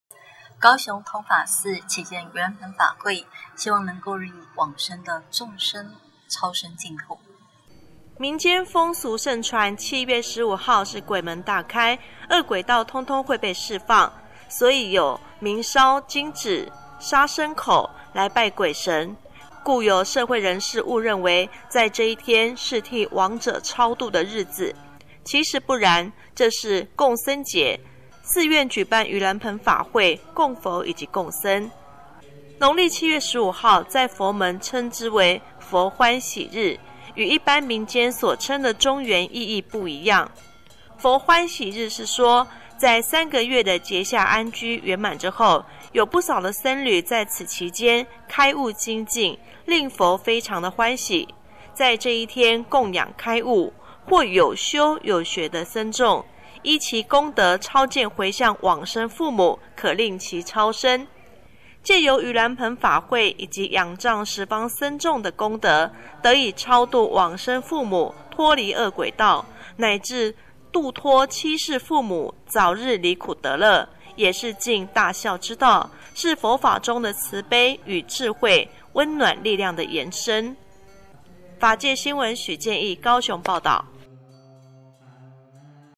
法界新聞》20150907通法寺舉辦盂蘭盆法會
法界新聞》
高雄通法寺舉辦盂蘭盆法會
透過誦經 、持咒的功德
讓往生親眷超生淨土